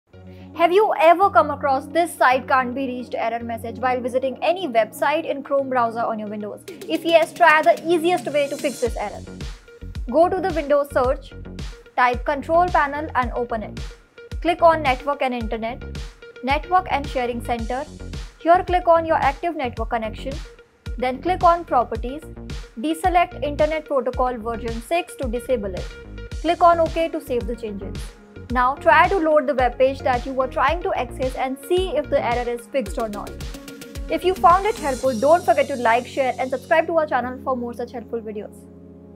How to Fix This Site Can't be Reached Error | This Site Can't be Reached Problem Solved
Once in a while, you may have run into “this site can’t be reached” while surfing a website on Google Chrome. The error can be very frustrating and you may want to know how to fix this cannot be reached. If Google Chrome Browser is unable to load a particular website, it displays “This site can’t be reached ” Error message.  If you’ve come across different errors, there’s a chance you might have faced this error: “This site can’t be reached” on Google Chrome, as well. In this video we will show you how you can solve the “This site can’t be reached” error.